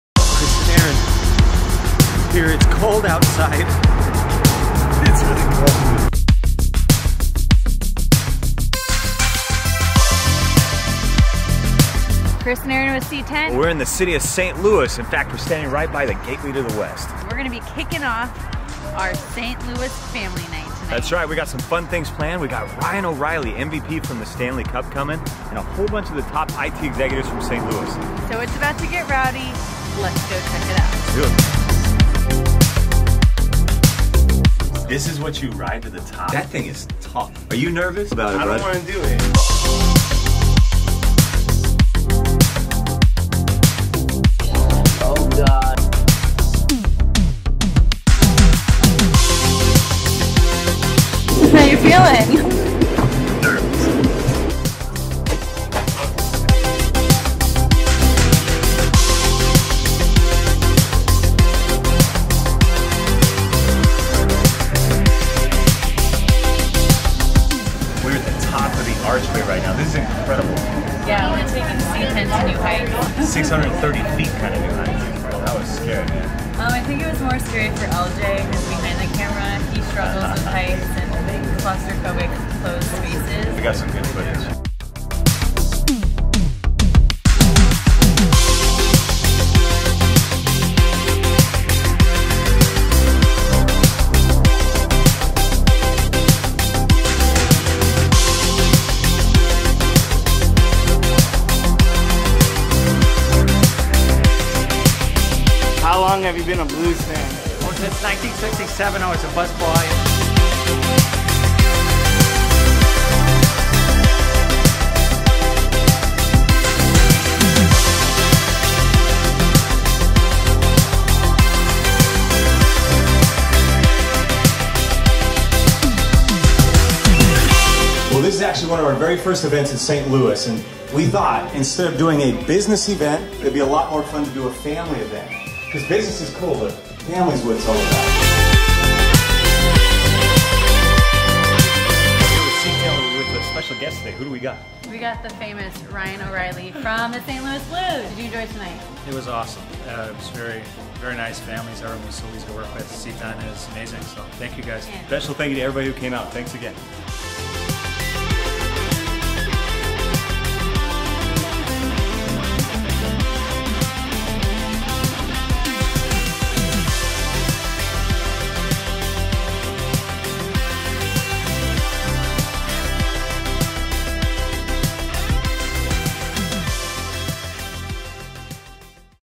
C10 VIP Family Night - St. Louis - Ryan O'reilly
Ever ride to the top of the St. Louis Arch before hosting a major event? We did! St. Louis is not only home to The Arch, but also the most recent Stanley Cup hockey team and one of the top IT Executive communities in the country. We brought Conn Smythe winner Ryan O'reilly to host our VIP Night with top trusted IT partners. Everyone was allowed to bring the family for this one and we had an awesome time getting to know everyone. Ryan signed a ton of autographs and took plenty of pictures with guests. It's always a great experience getting some time with a current champ.